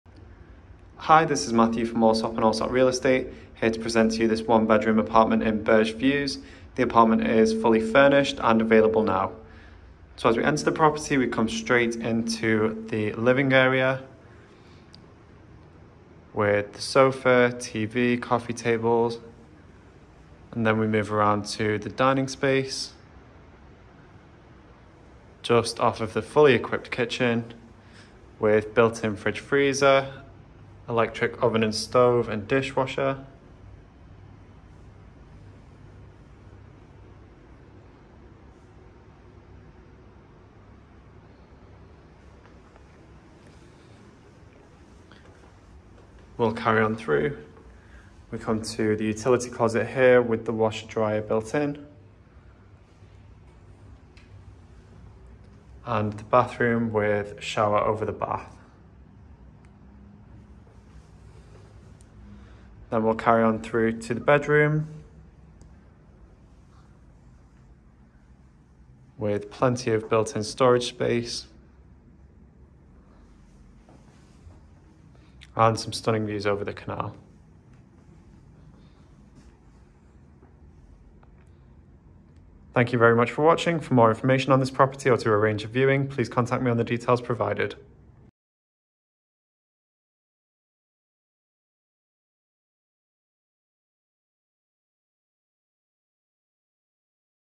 Available immediately: Fully furnished 1-BR with chiller-free in Downtown Dubai!
Fully Furnished One Bedroom Available For Immediate Rent

- One Bedroom
- Vacant
- Fully Furnished
- High Floor
- Natural Light
- Pool and Gym Access
- One Parking
- Chiller Free
- Reference: L-206662

Allsopp & Allsopp are pleased to present this well maintained one bedroom in Burj Views. A good-sized open space and fully equipped kitchen, a square living area and dining area to the side of the kitchen. To the left, you have a laundry room, bathroom along with a bright bedroom with ample built-in storage.